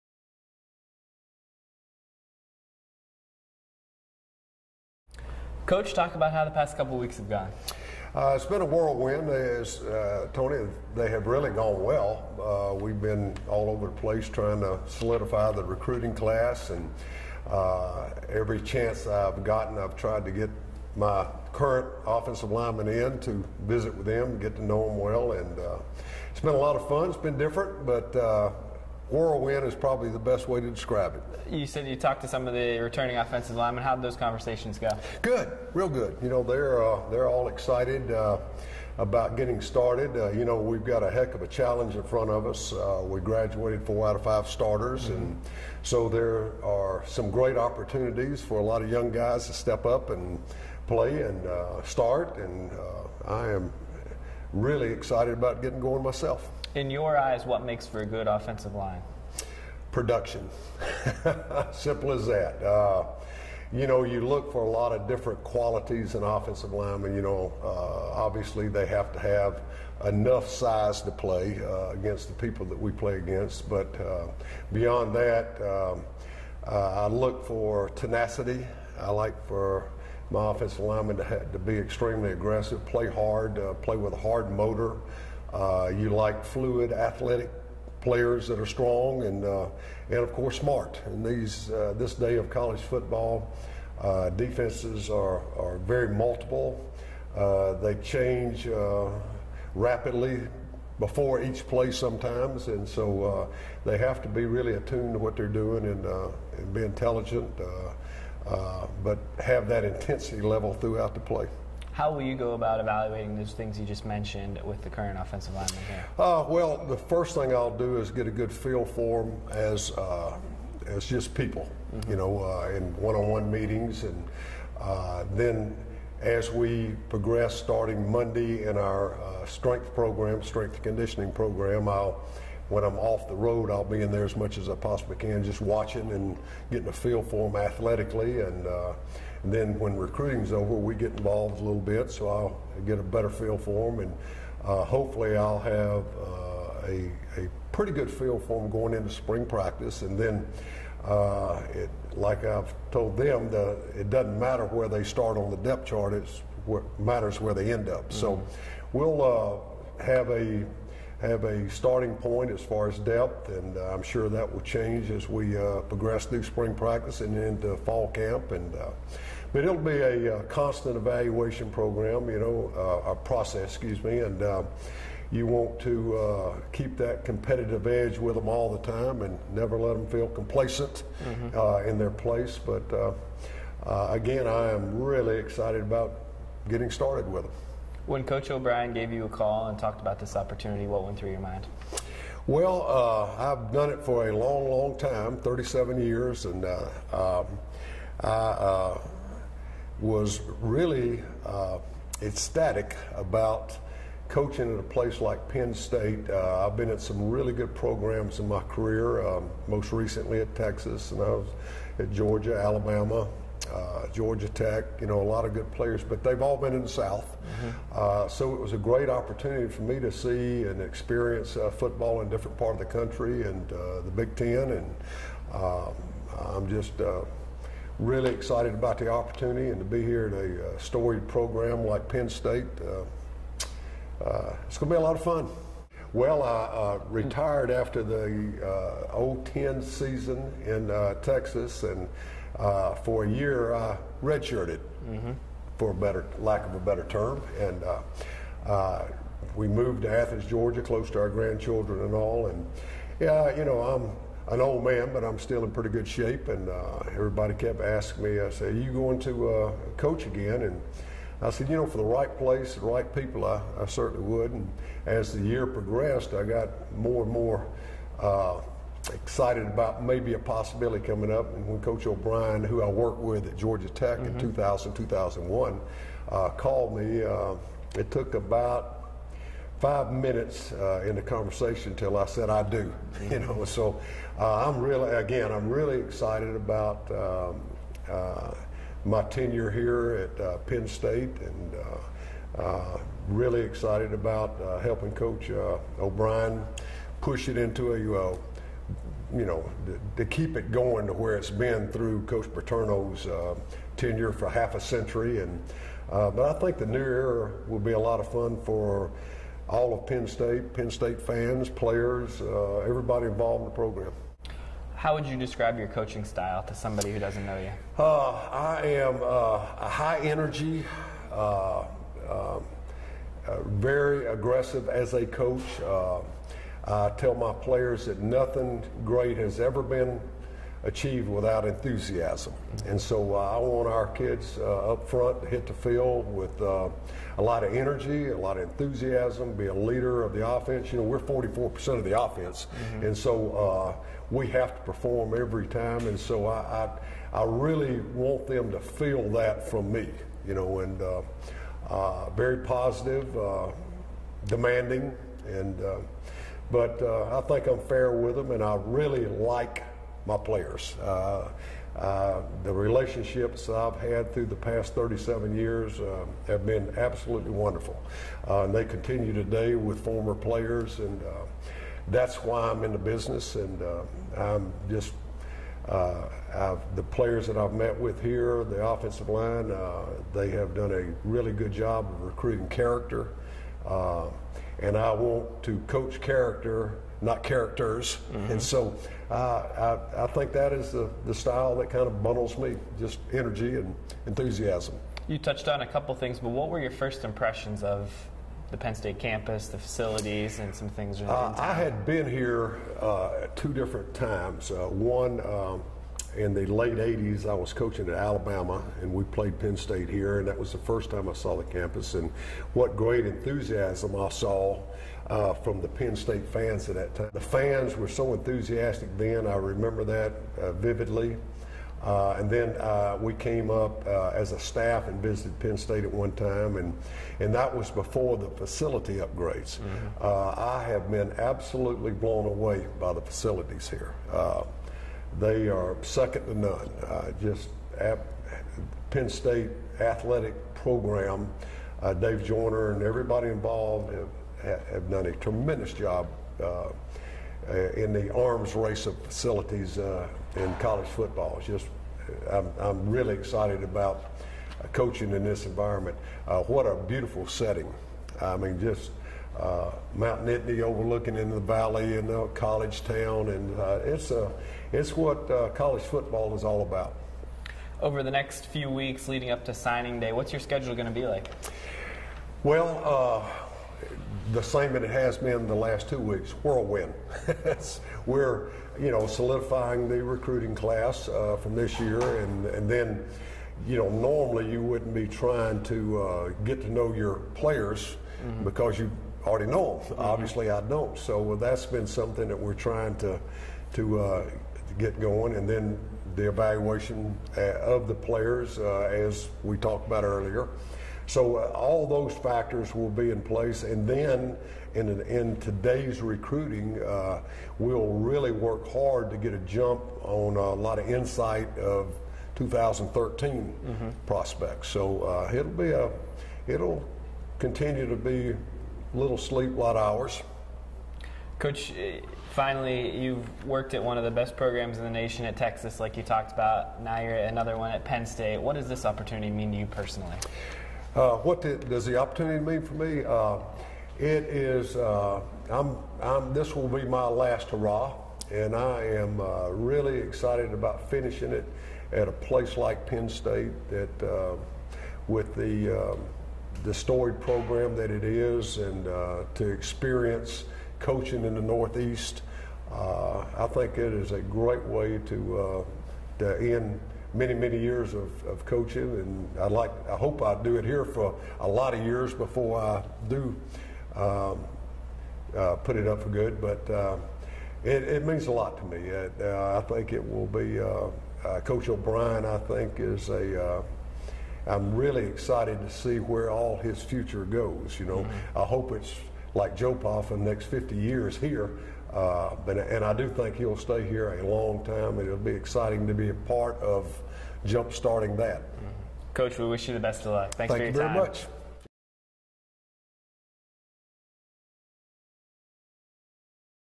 Exclusive One-on-One with Penn State Assistant Mac McWhorter
GoPSUsports.com's Tony Mancuso talks with new Penn State Offensive Line Coach Mac McWhorter.